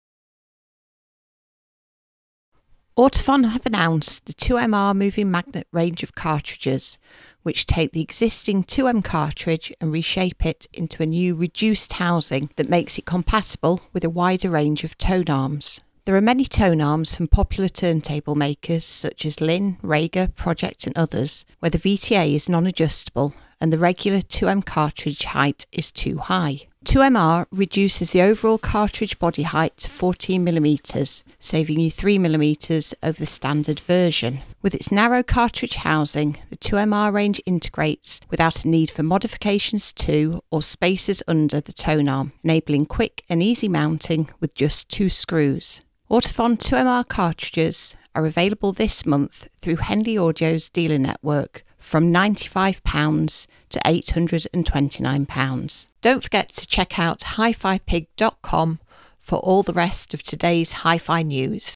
Ortofon 2MR Cartridges For Vinyl Record Players and Tonearms
Ortofon the cartridge manufacturer, have announced the Ortofon 2MR Moving Magnet range which takes the existing 2M cartridge and re-shapes it into a new “Reduced” housing that makes it compatible with a wider range of tonearms.